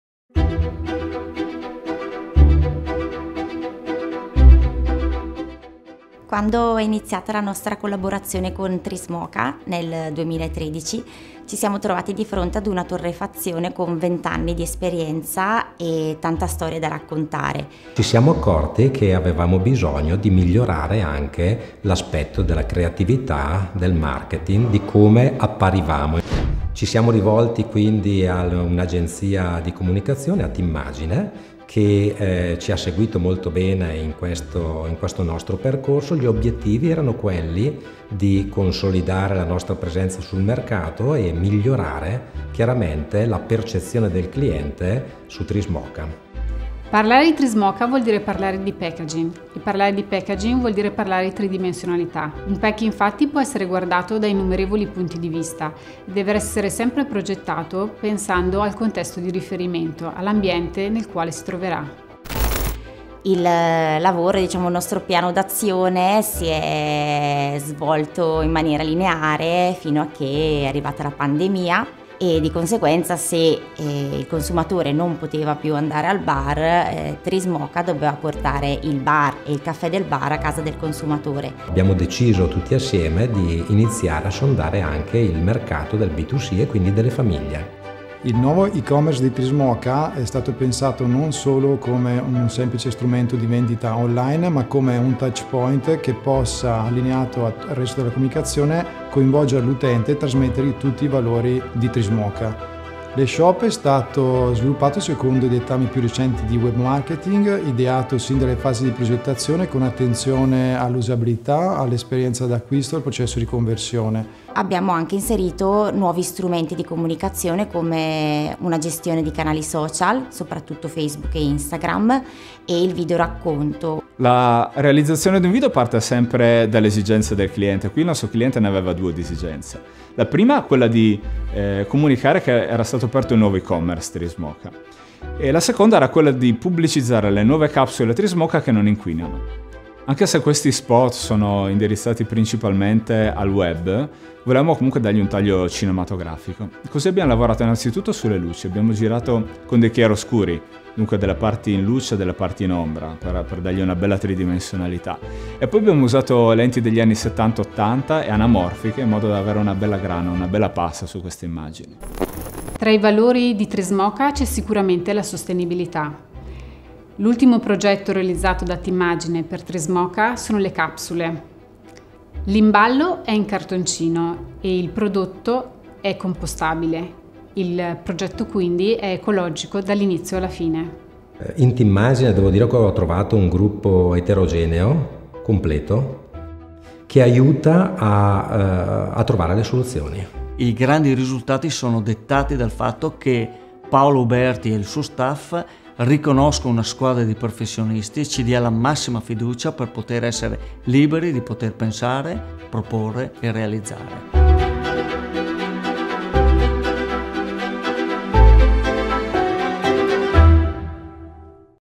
Timmagine - Case History: Trismoka
Dal 2013 Trismoka si appoggia ai professionisti di Timmagine  nella realizzazione della propria mission, la diffusione della cultura del buon caffè. Attraverso attività di social media marketing, ufficio stampa e advertising la nostra agenzia ha lavorato con strategia e creatività per offrire al cliente un servizio di comunicazione promozionale e informativa, sempre all’altezza dell’eccellenza che contraddistingue il caffè Trismoka.

Dalla volontà di allargare un buisness tradizionalmente B2B anche ai consumatori è nato il nuovissimo e-commerce Trismoka, creato da Timmagine insieme a una campagna promozionale che offrirà una spinta preziosa al successo dell’e-shop.

Da nuove esigenze, nuove soluzioni.